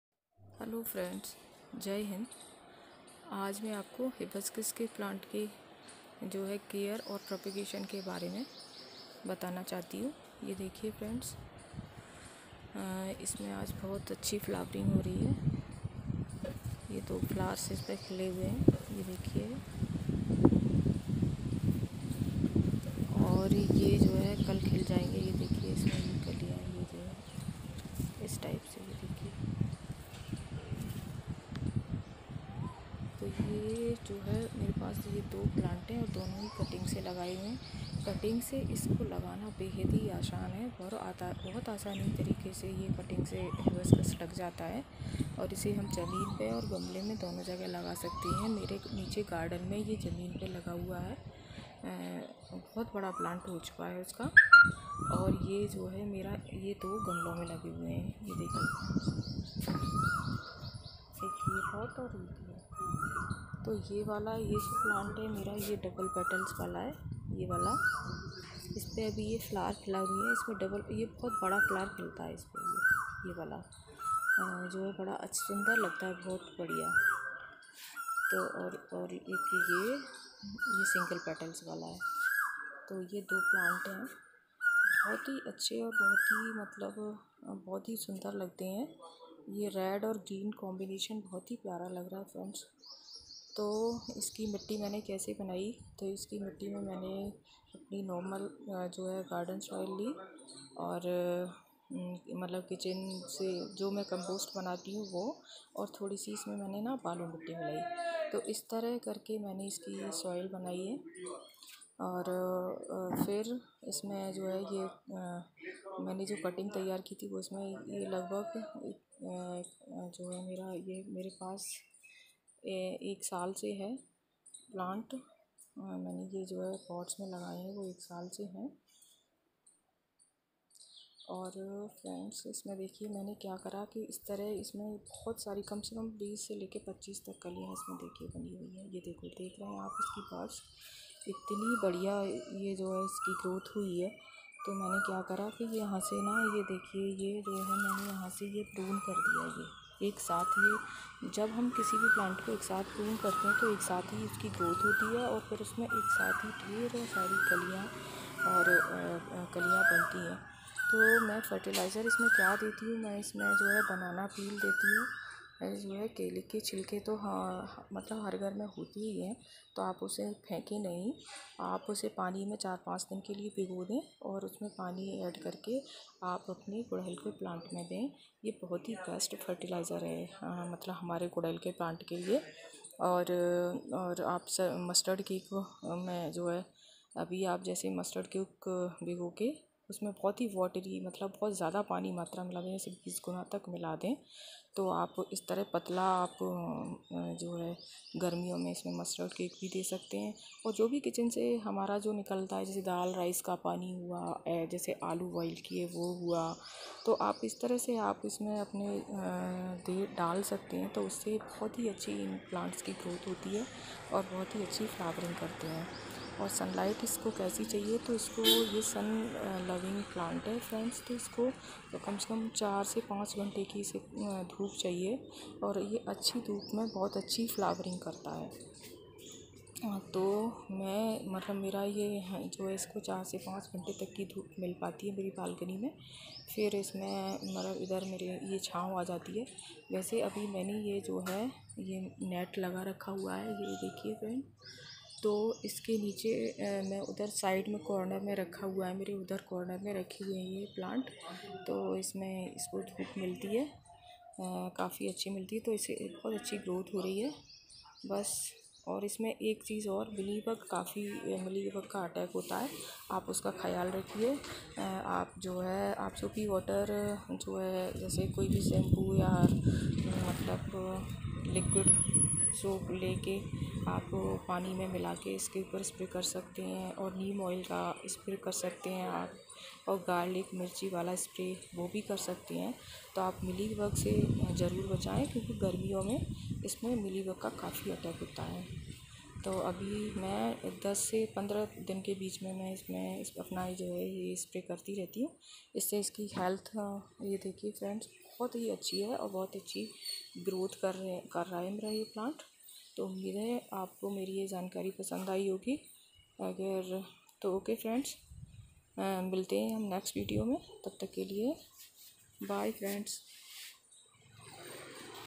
गुडहल पर ज्यादा फूल पाने के तरीके // get more flowers on hibiscus plant tips and tricks 🌿🌺🌺🌿🌿
Maine is video mai hibiscus plant ki care  ke bare mai bataya hai